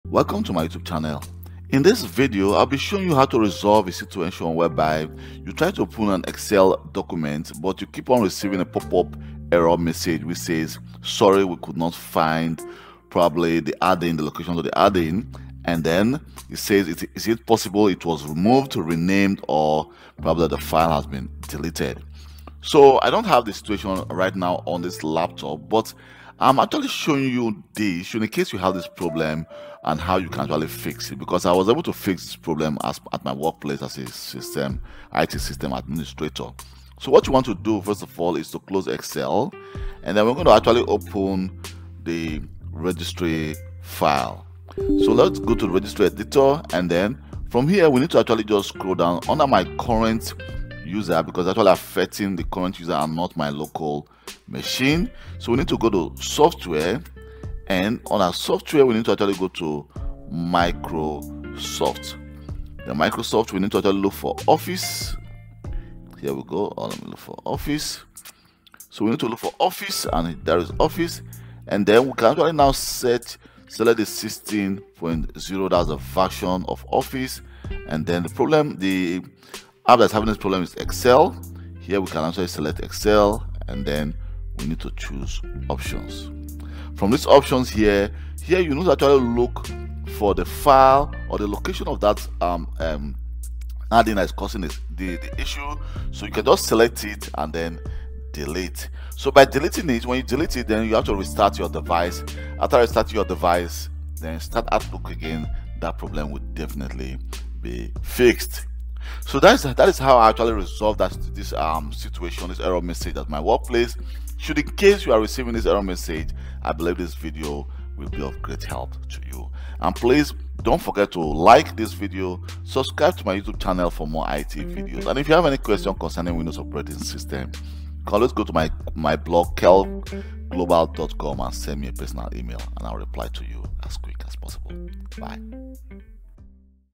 Fix - Sorry We Couldn’t Find Your File Was It Moved Renamed Or Deleted Error? Fix Excel error issues
When you open an excel file and the receive the following error message Sorry, We Couldn’t Find Your File Was It Moved Renamed or Deleted Error?
This video will be a quick guideline on how to resolve the issue.

Looking to elevate your IT skills to the next level? Check out this amazing course on mastering IT Systems Administration & Azure Cloud Engineering by Udemy!